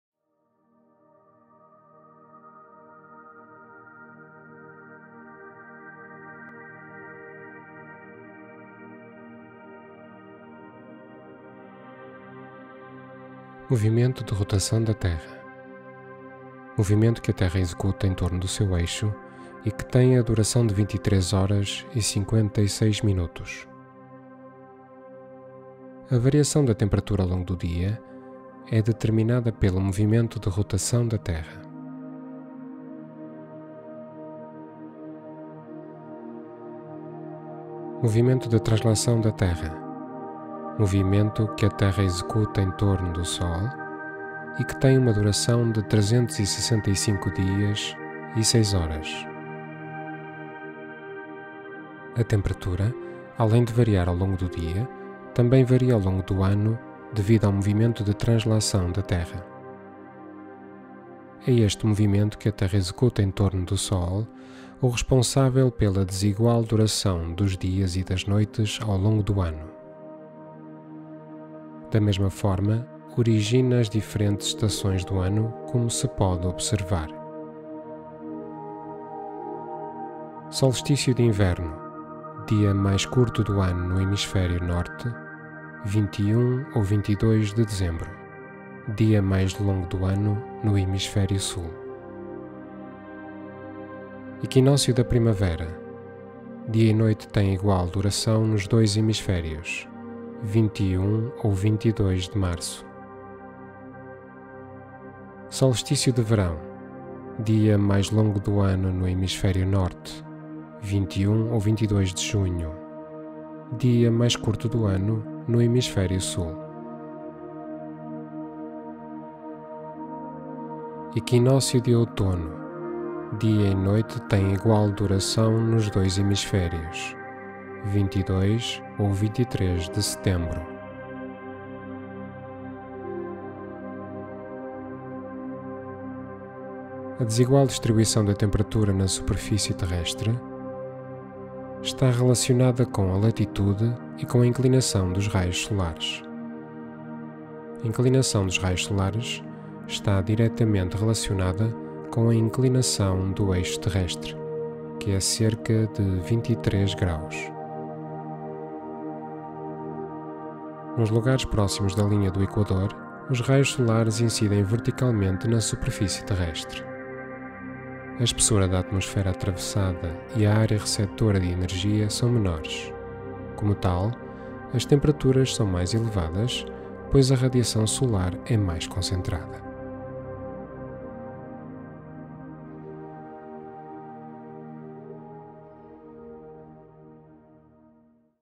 A distribuição da temperatura na Terra e os fatores condicionantes
Geografia 7º Ano
Tema: O Meio Natural
Subtema: O clima e as formações vegetais
Conceitos: Equinócio, solstício, movimento de translação, movimento de rotação